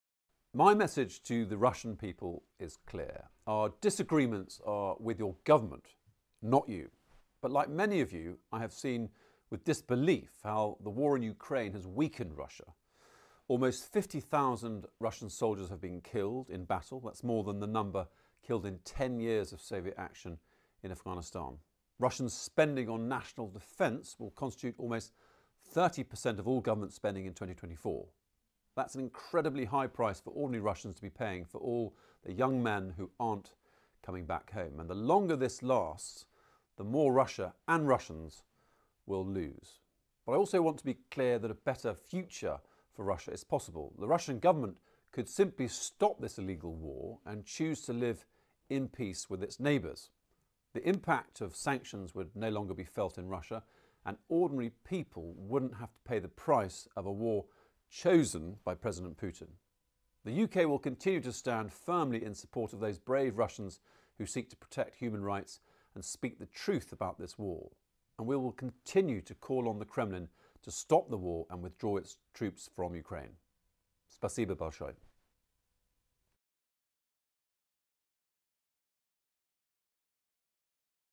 Обращение Министра по делам Европы Великобритании Лео Дохерти к россиянам
Обращение Министра по делам Европы Великобритании Лео Дохерти к россиянам: "Лучшее будущее для России – возможно."